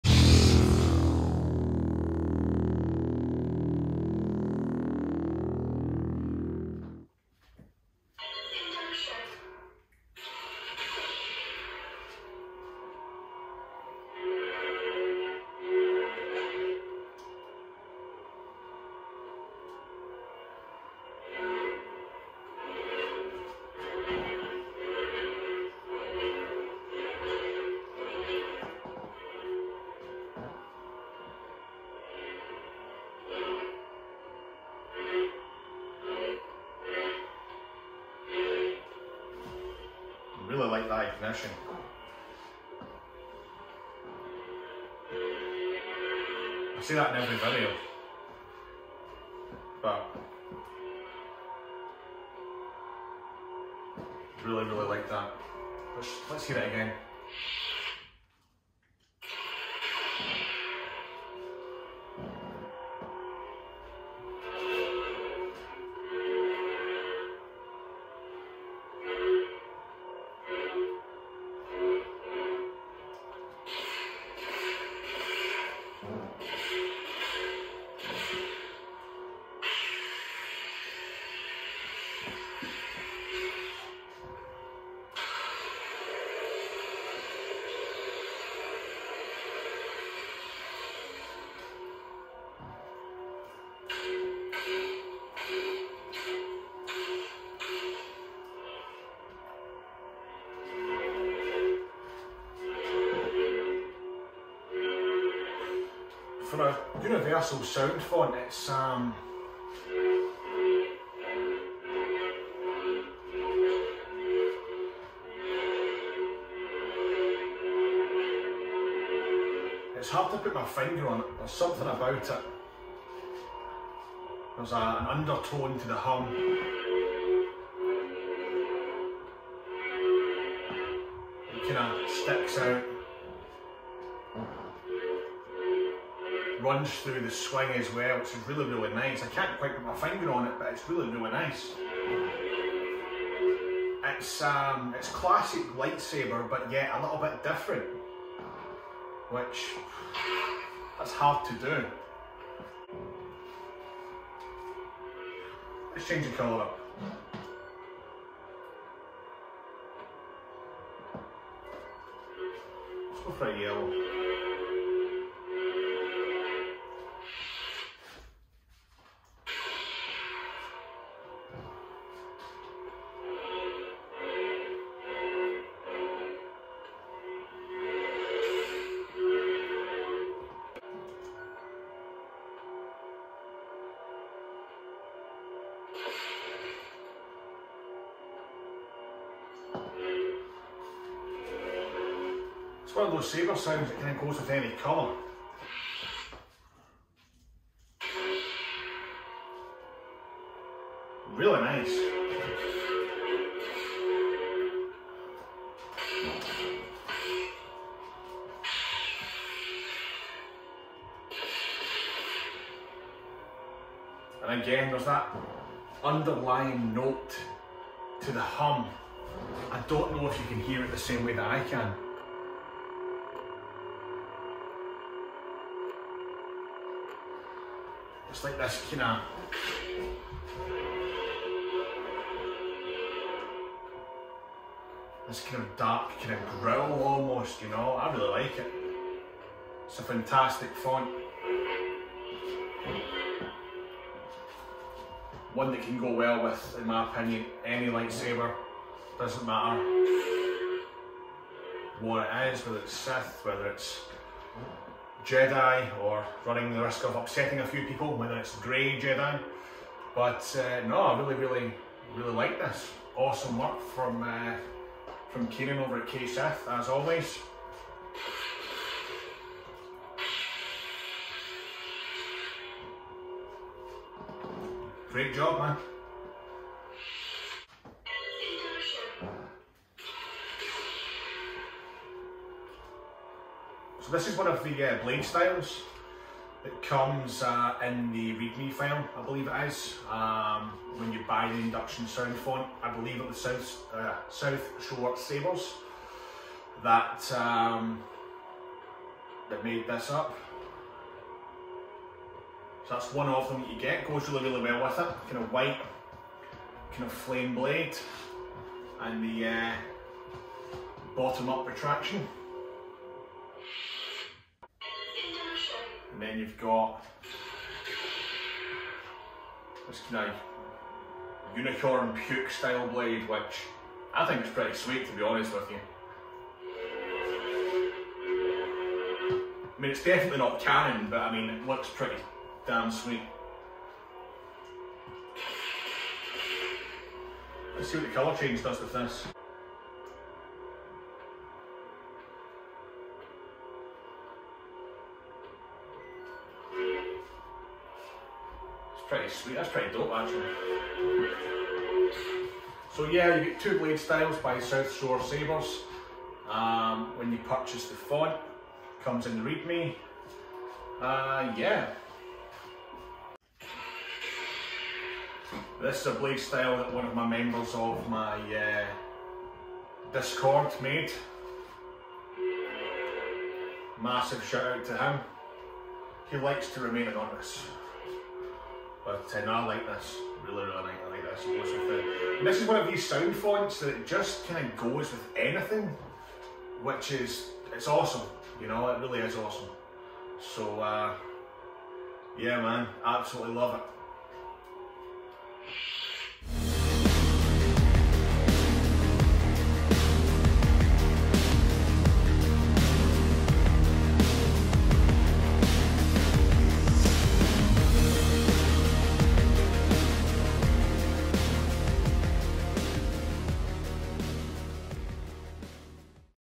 Induction By KSITH (4K) | Universal Proffie Neopixel Lightsaber Sound Font | CFX | Verso | Asteria
Demo of Induction by KSith.

In KSiths own words, "Modern, Industrial, futuristic, and powerful". That traditional saber sound we all know & love, with an added drop of awesomness!

Perfect for ANY saber!

I am using this on a Proffie Sound Board but you also get the files for CFX & Verso when you download.

*LINKS*

CHEAP LIGHTSABER  WALL MOUNTS

*LINKS TO TRUSTED COMPANIES*

*LINKS FOR SOUND FONTS*